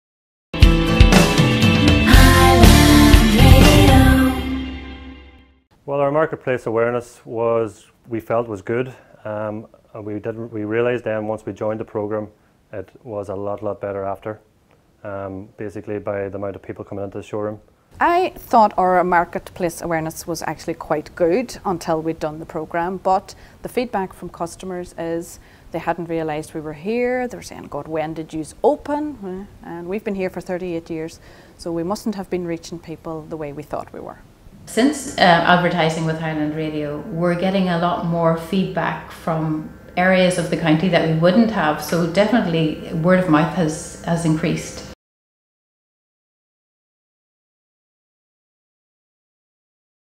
Advertise on Highland Radio
Highland Radio customers give their thoughts on the benefits of advertising on Ireland's Number One Local Station.